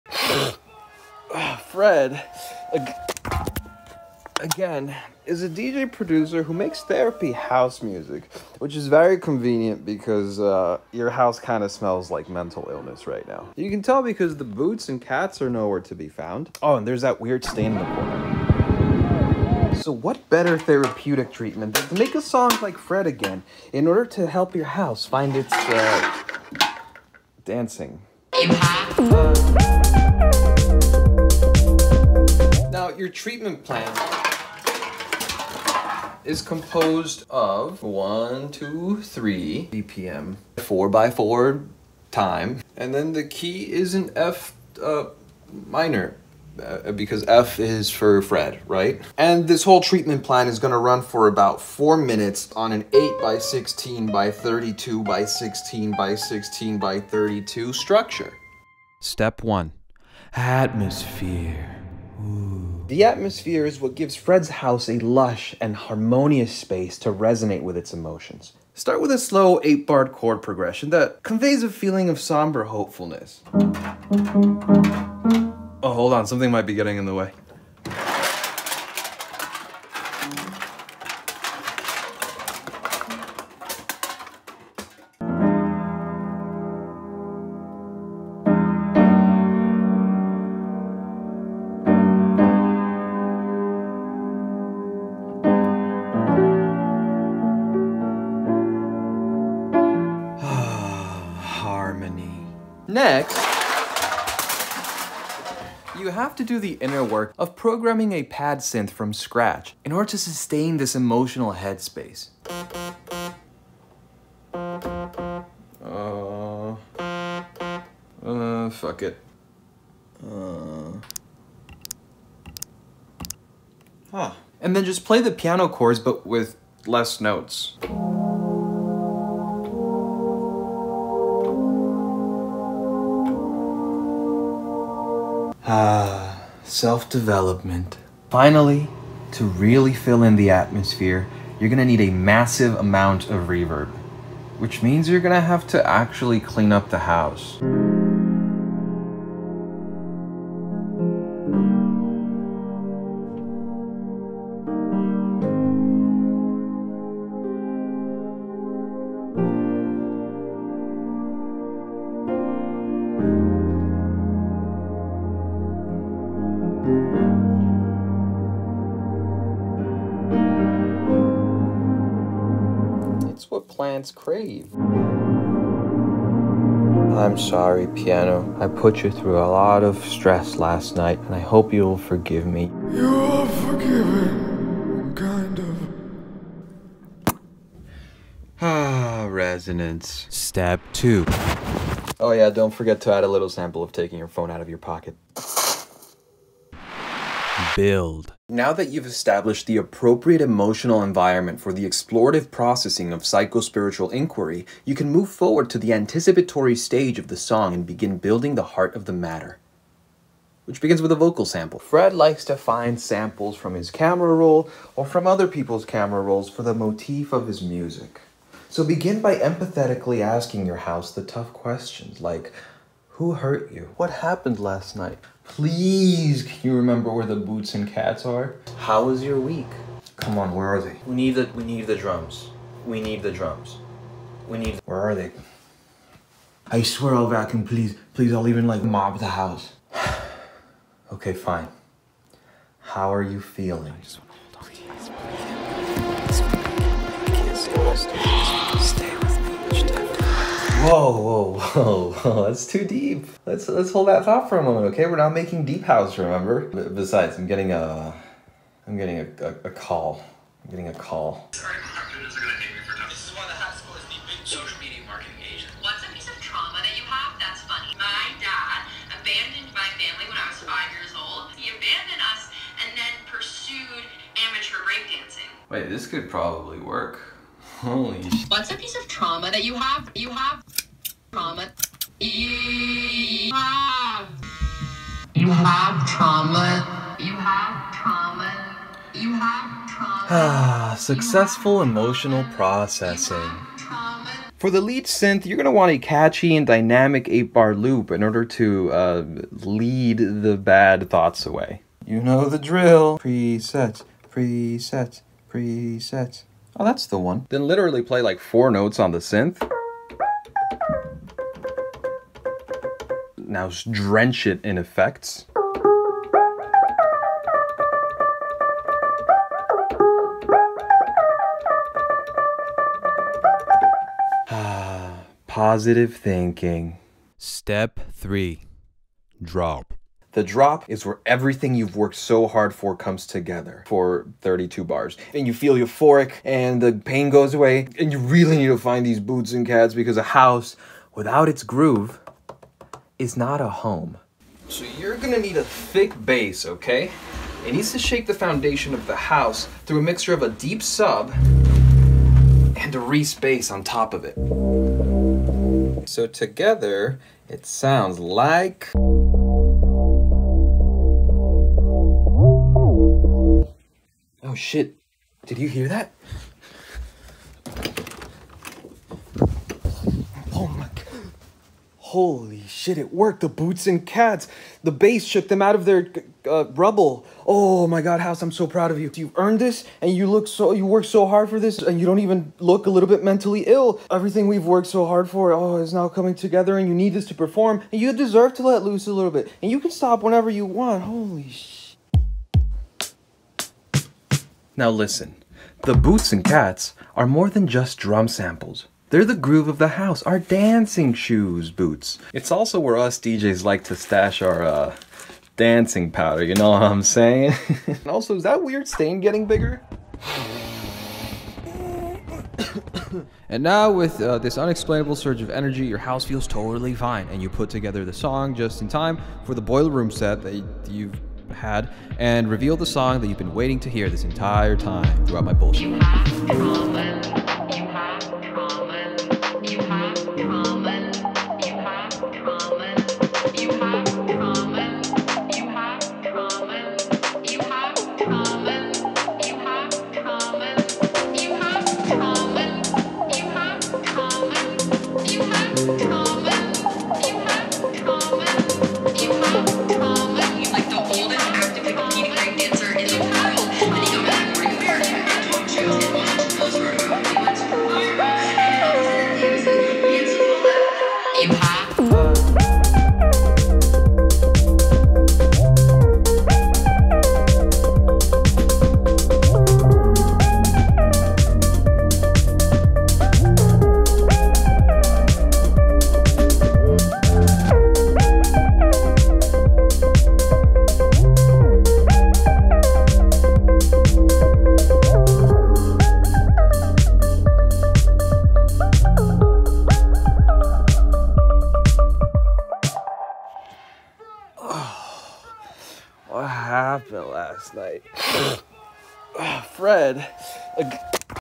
How To Fred again..
House feeling mentally ill? Lost your "Dancing" essence? Then produce a track like Fred again.. because of its therapeutic house properties (but try not to dance TOO hard ykwis).